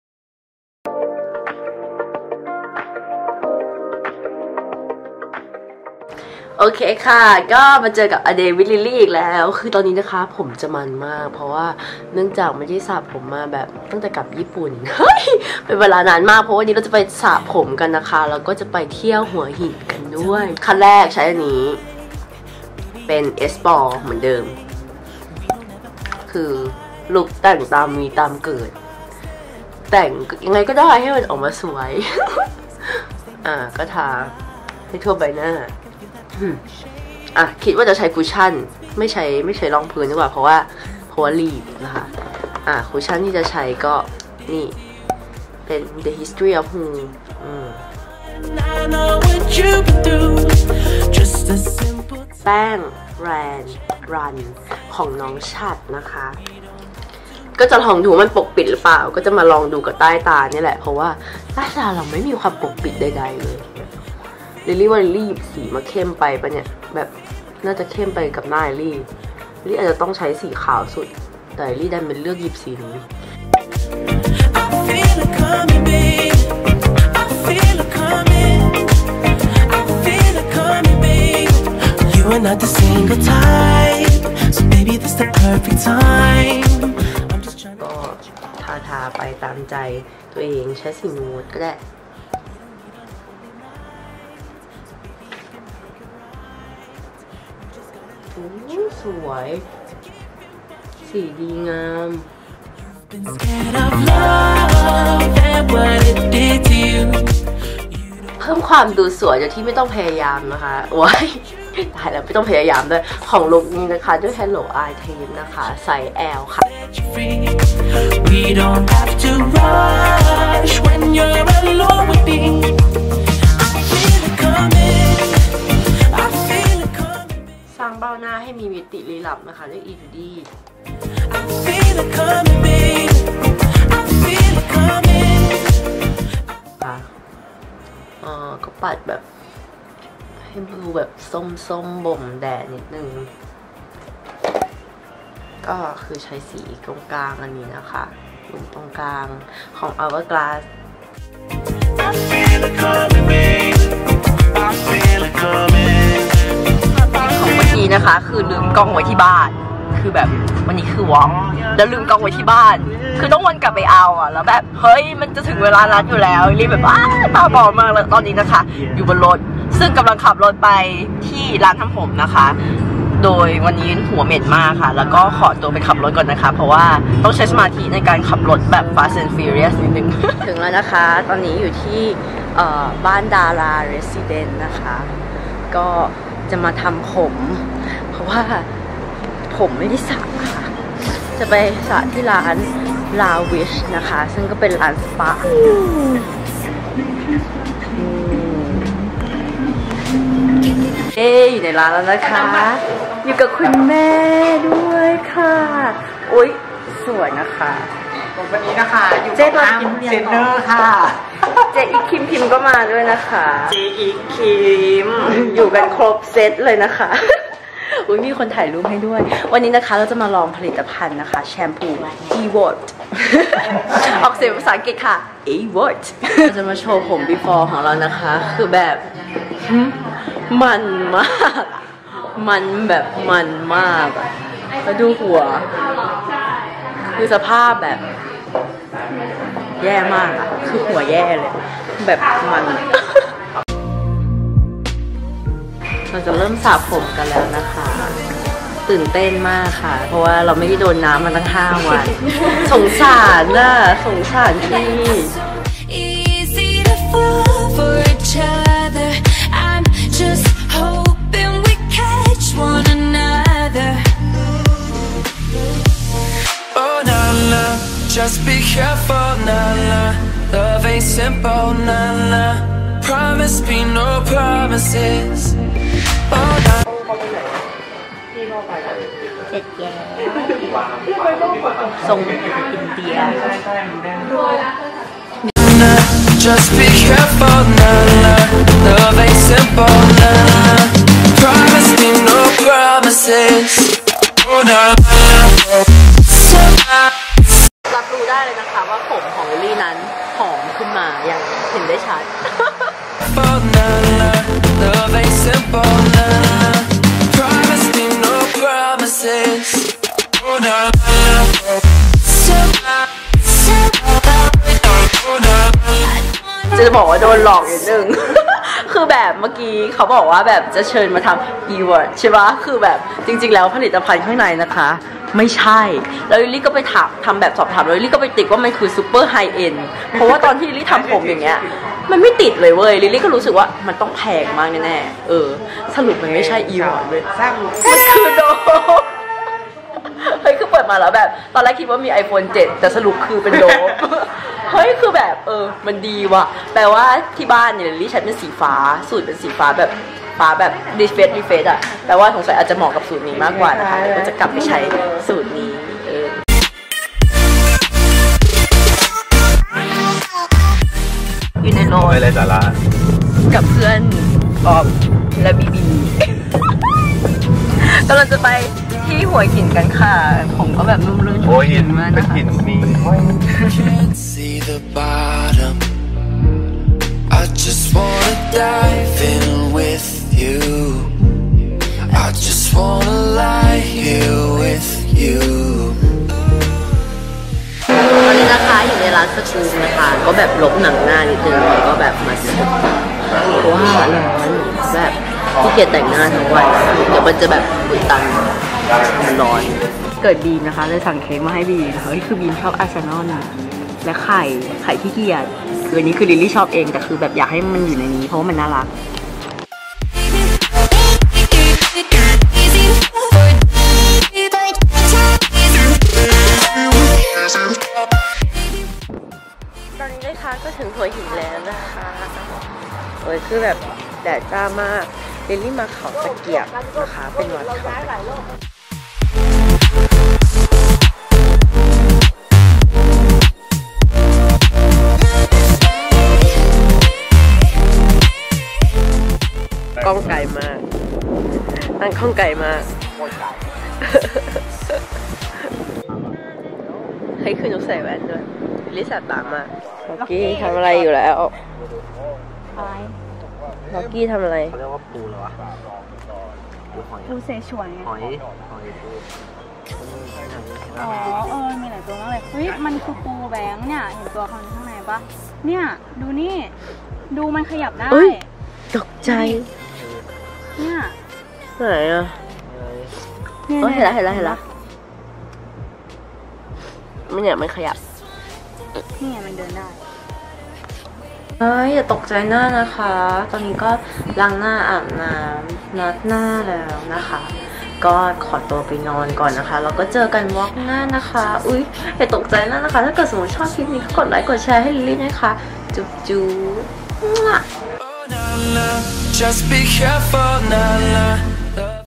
A day with Lily EP.9:แต่งหน้าไปโดนหลอก!+เที่ยวหัวหินวันเกิดบีม
เข้ามาทักทายกันได้ตามช่องทางนี้เลยค้า
Hope you like this video:D